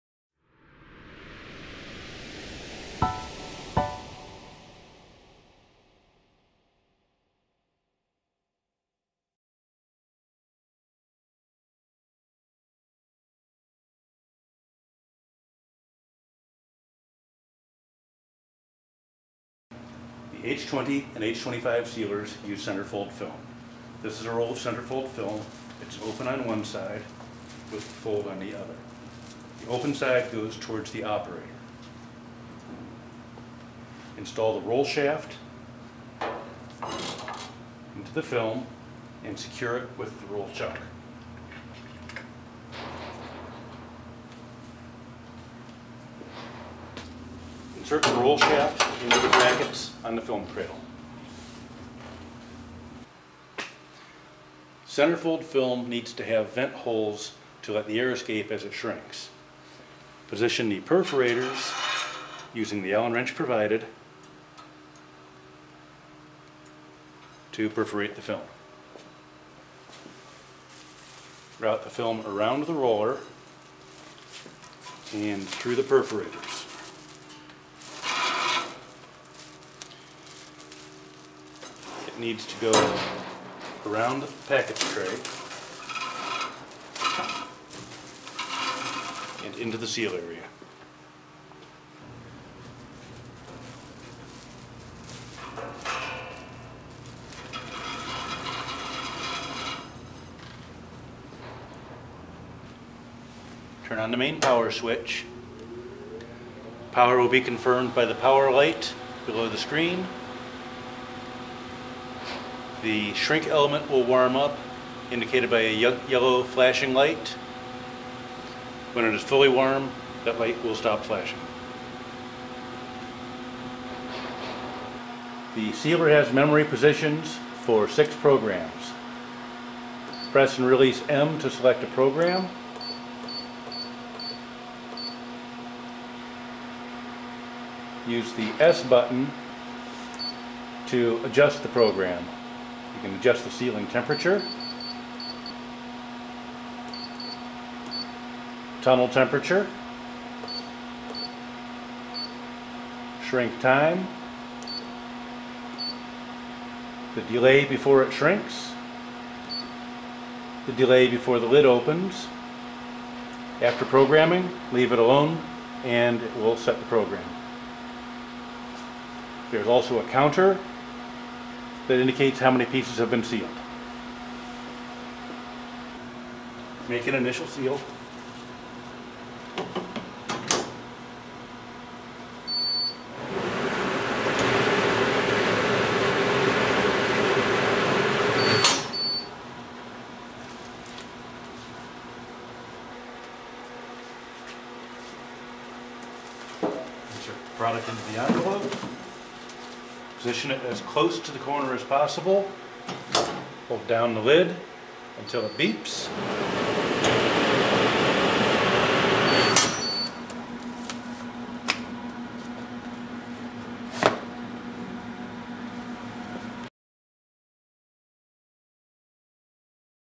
Dempack H20 and H25 One Step Shrink Systems Tutorial
Learn more about the Clamco Dempack H20 & H25 One Step Shrink Systems and many more shrink wrapping machines at spiralbinding.com

The Dempack™ H20 & H25 are one-step combination seal / shrink  systems that operate with a shrink hood instead of a tunnel. The hood is closed by an electronic hold-down magnet during the film sealing and heat-shrink wrapping operations.